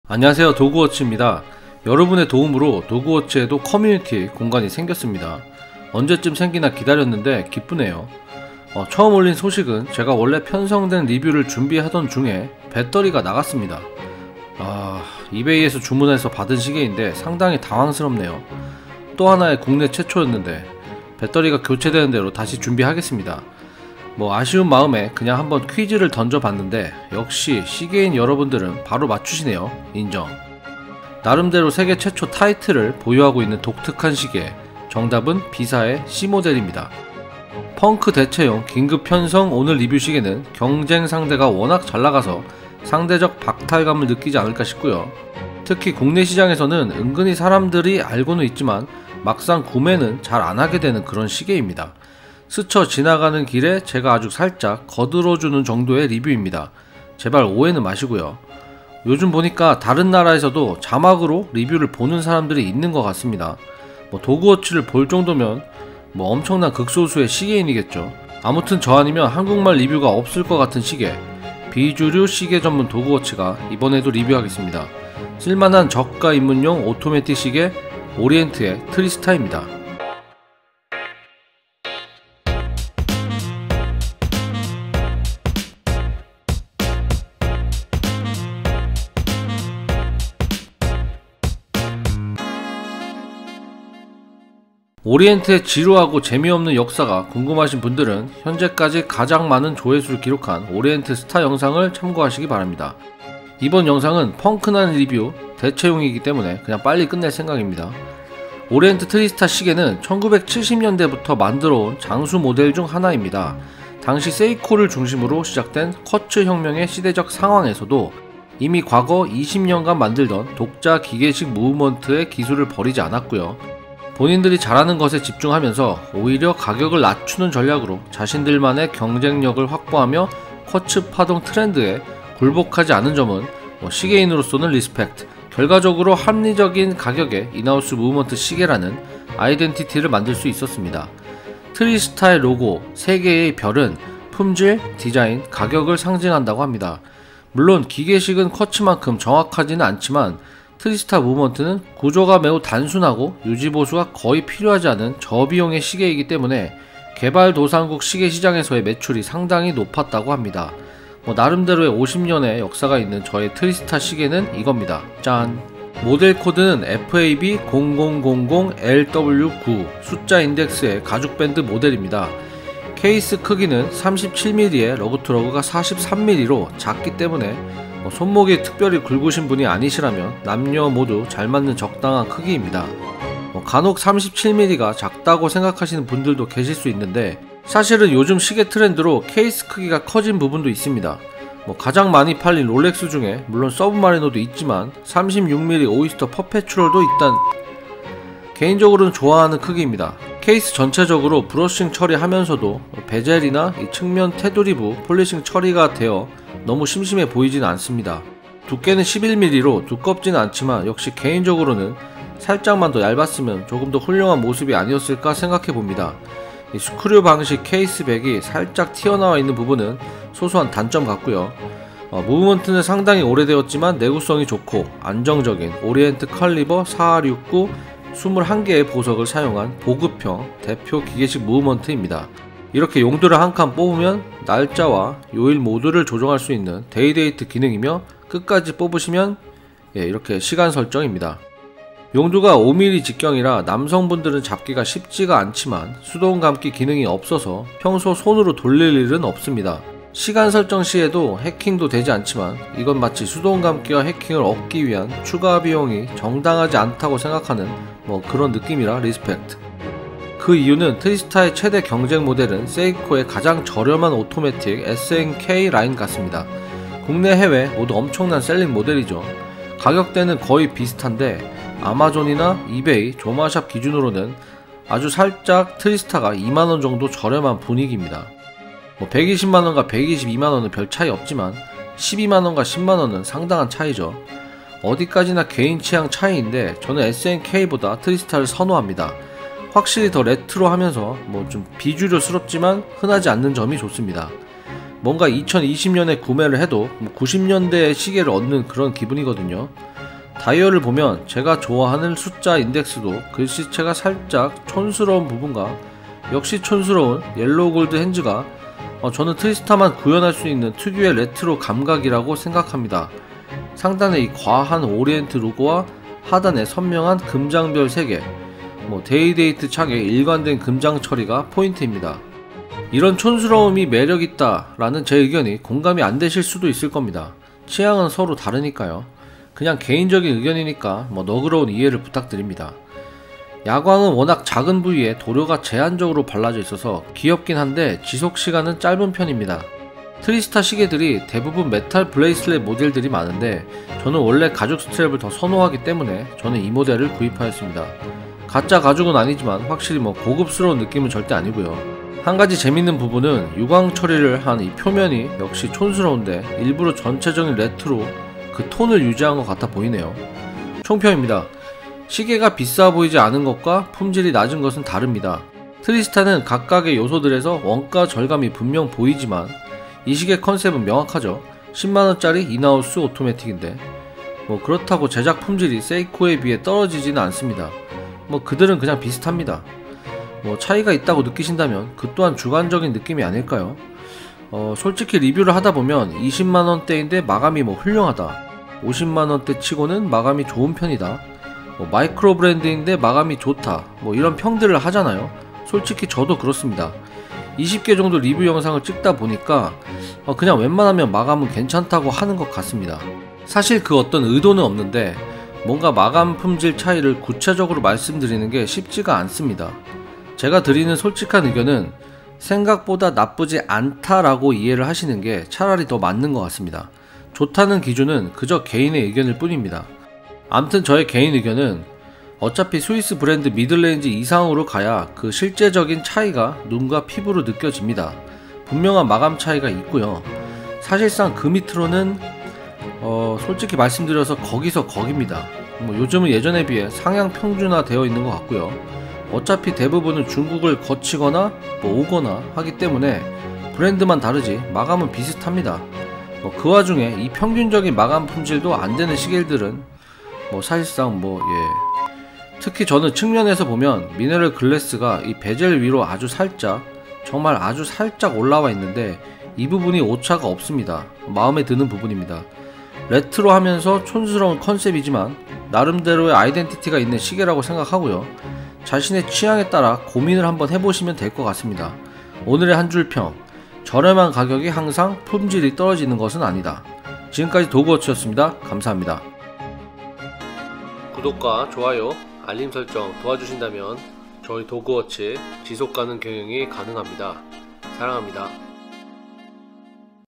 [도그워치 리뷰#18] 저렴한 레트로 오리엔트 트리스타 Orient TriStar 싸고좋은 오토매틱 10만원
원래 리뷰중이던 시계가 문제가 생겨
긴급 편성하여 올립니다. 이번 리뷰는 오리엔트 트리스타
여러가지 이유로 호불호가 있지만
저렴하고 품질좋은 인하우스 기계식 무브먼트
촌스러움에 매력을 느끼시는 분들에게?
드리는 맞춤리뷰입니다^^